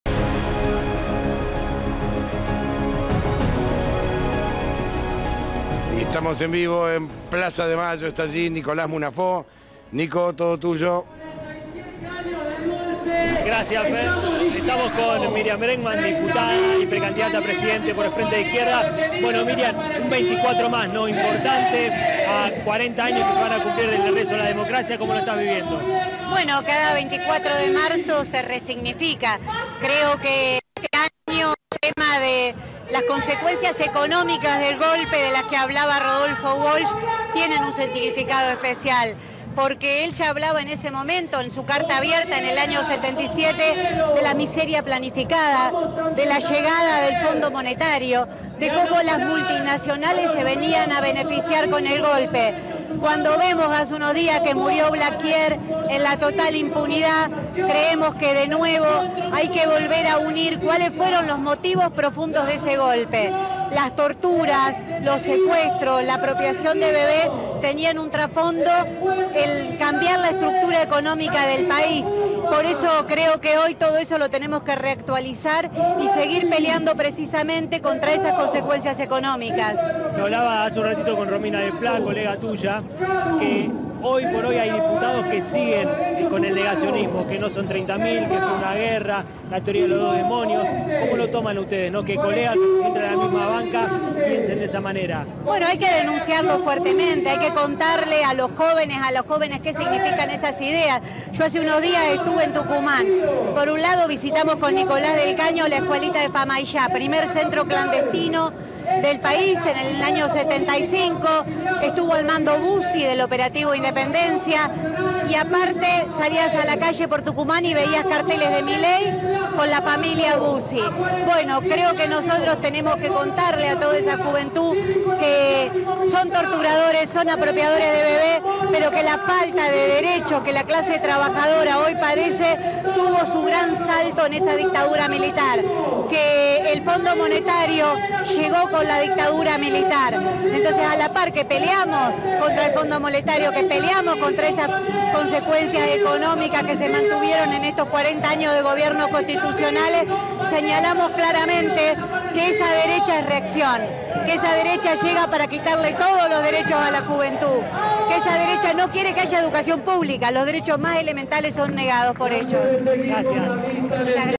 Myriam Bregman "El FMI llegó con la dictadura militar y las consecuencias las seguimos pagando"
La precandidata a presidenta dialogó desde Plaza de Mayo con @c5n